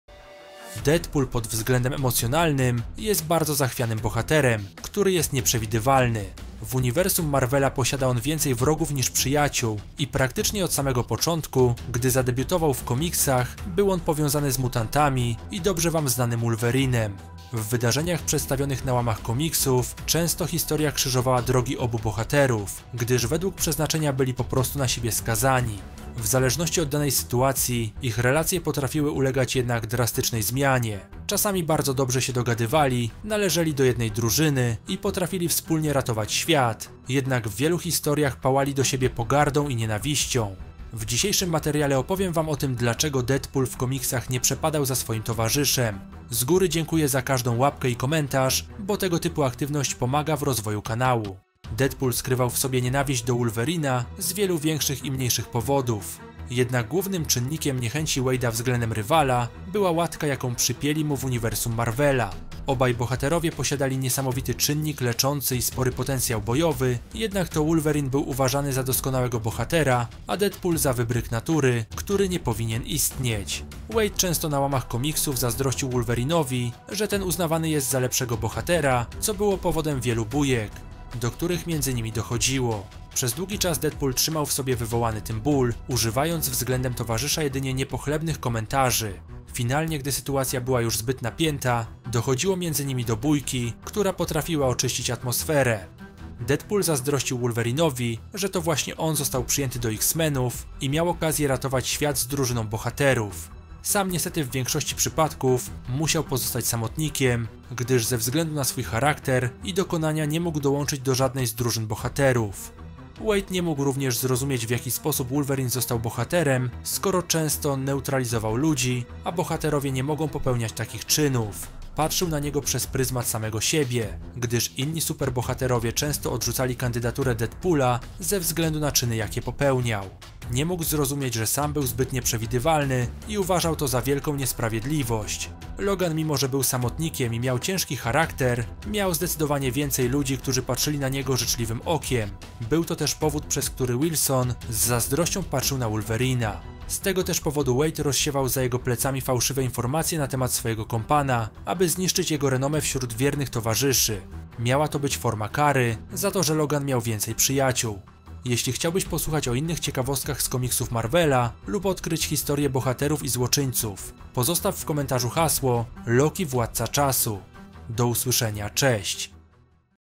Dlaczego Deadpool Nienawidzi Wolverina?
Czy Deadpool czuje do Wolverina urazę? Dlaczego? Czy to jego kompleksy?